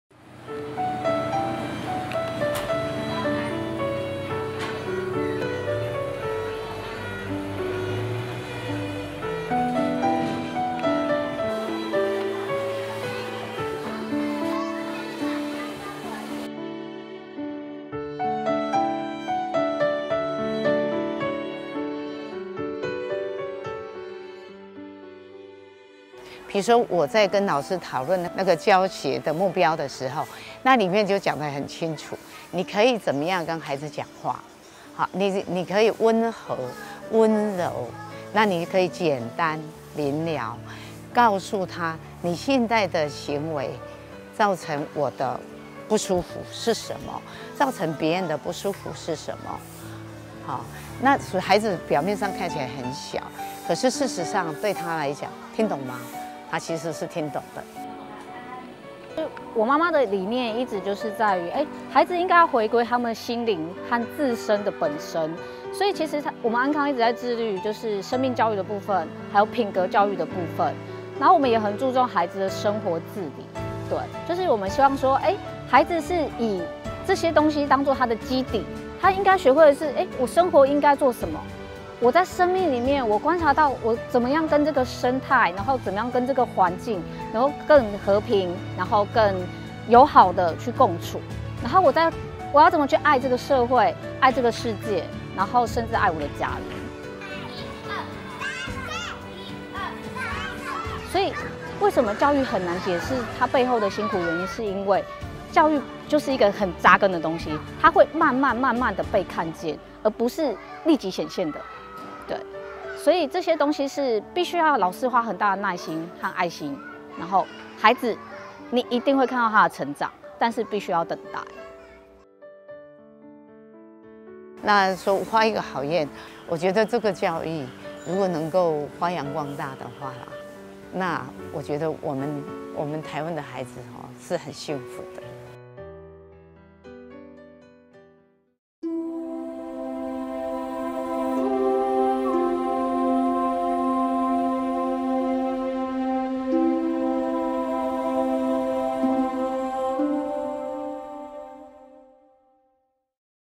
【好願分享】擁愛教育—傳承師道的慈光
〈擁愛教育—傳承師道的慈光〉

媽媽傳承給我的理念，就是在於孩子應該回歸於他們的心靈和自身，所以我們幼兒園一直致力於生命教育及品格教育，然後我們也很注重孩子的…….

~幼兒園園長 吳玟慷